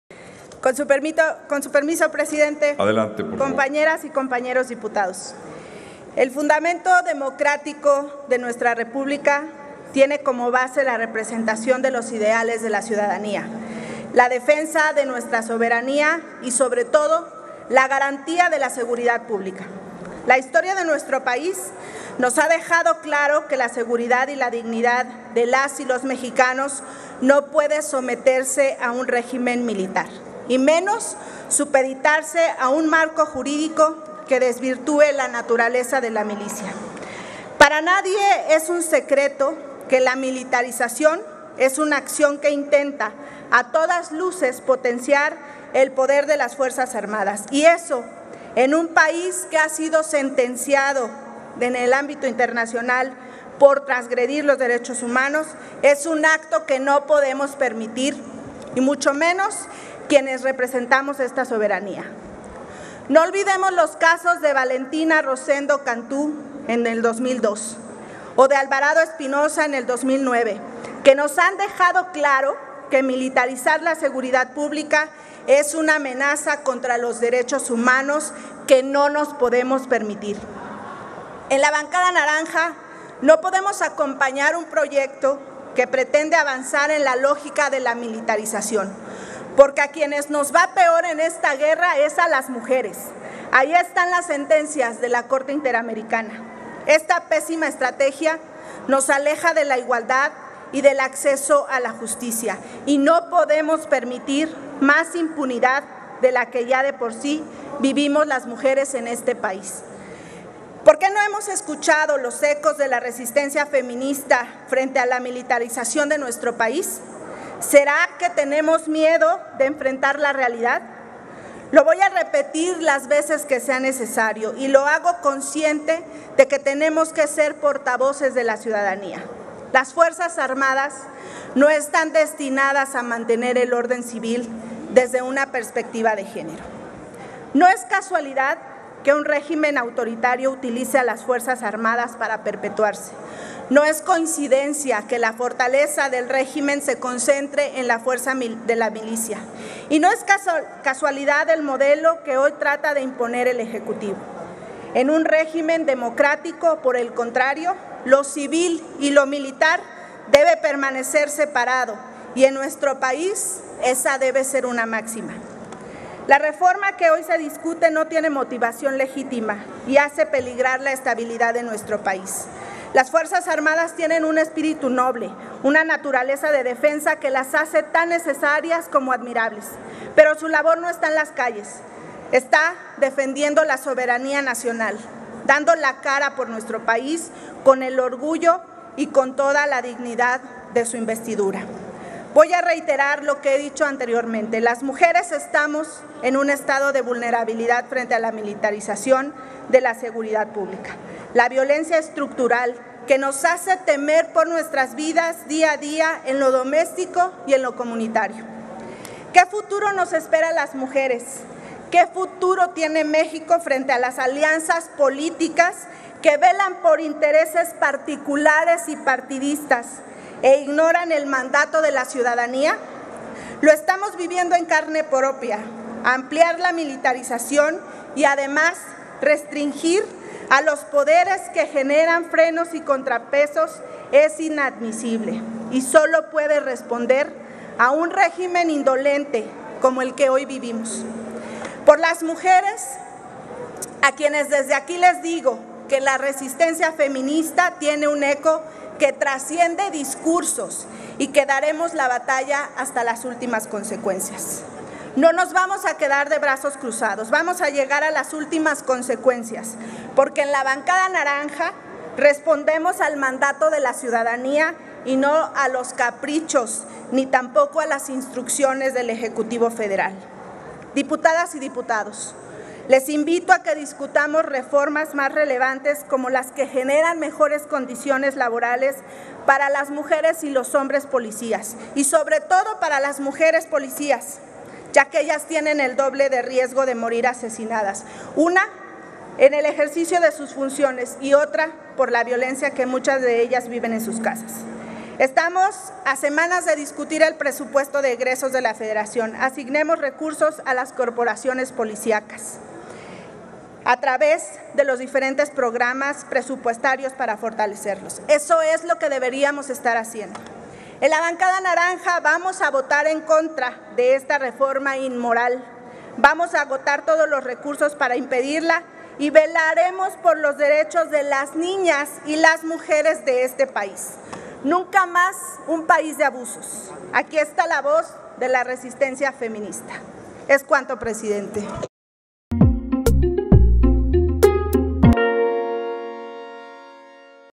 Dip. Jessica Ortega de la Cruz (MC) / En contra del dictamen en materia de Guardia Nacional.
La diputada Jessica Ortega de la Cruz (MC) afirma que esta soberanía no puede permitir que la seguridad y dignidad de las y los mexicanos se someta a un régimen militar, y menos que éste se sujete a un marco jurídico que desvirtúa la naturaleza de la milicia.